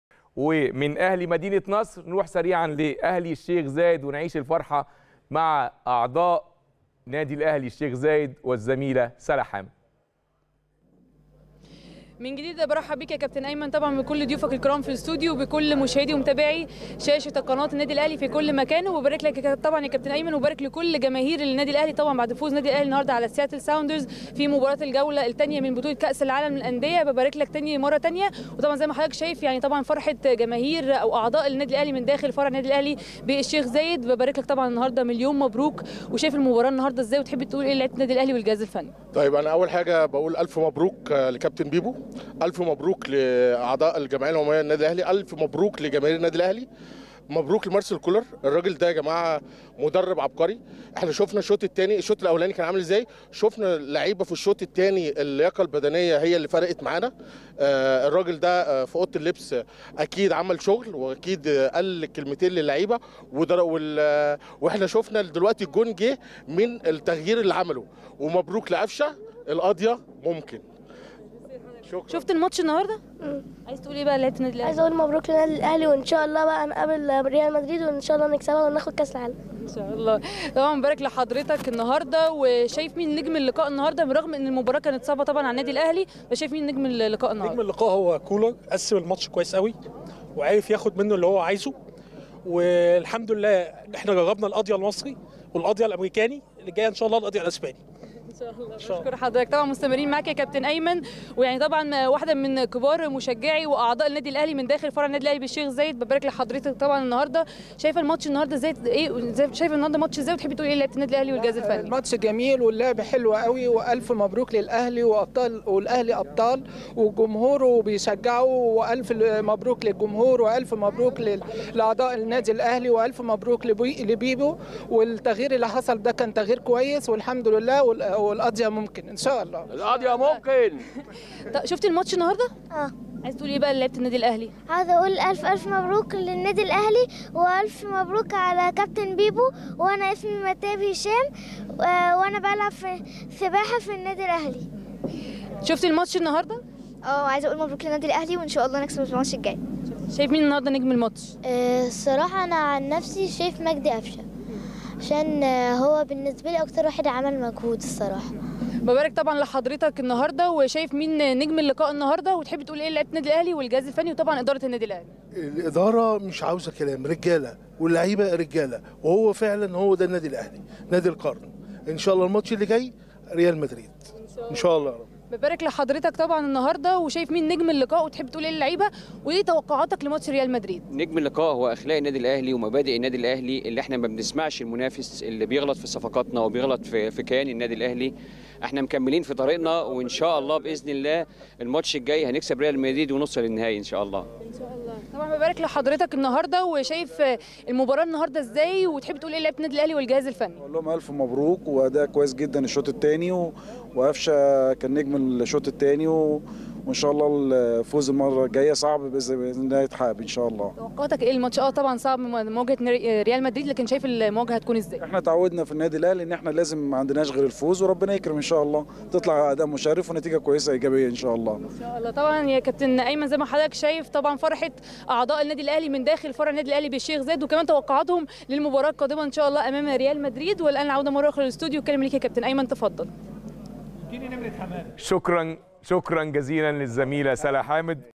سلا حامد 🎙 وفرحة عارمه مع اعضاء أهلي الشيخ زايد بعد التأهل لنصف نهائي المونديال
سلا حامد 🎙 وفرحة عارمه مع اعضاء أهلي الشيخ زايد بعد التأهل لنصف نهائي المونديال | الاستوديو التحليلي